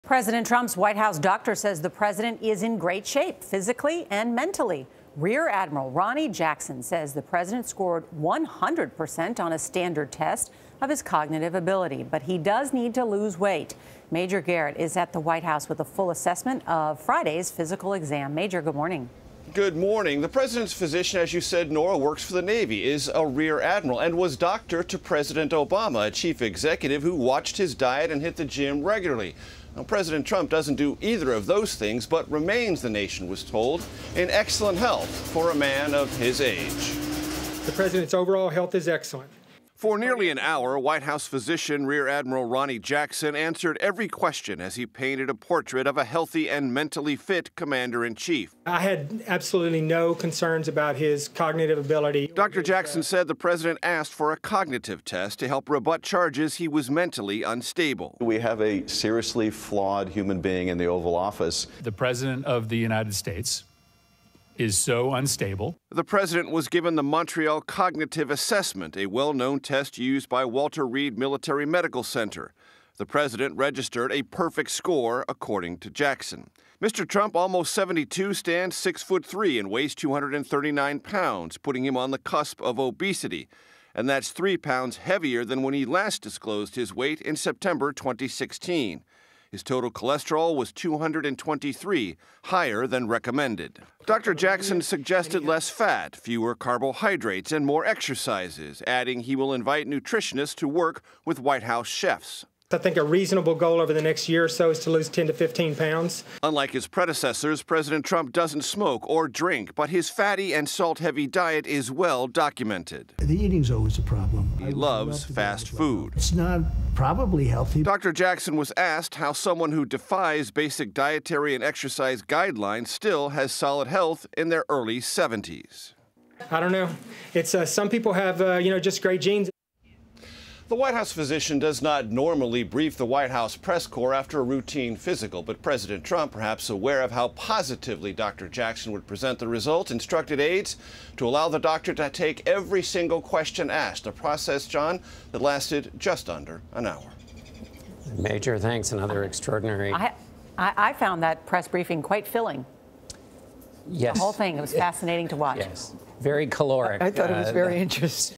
White House doctor deems Trump in "excellent" health
President Trump's White House doctor says the president is in great shape, physically and mentally. Rear Adm. Ronny Jackson says the president scored 100 percent on a standard test of his cognitive ability, but he does need to lose weight. Major Garrett reports.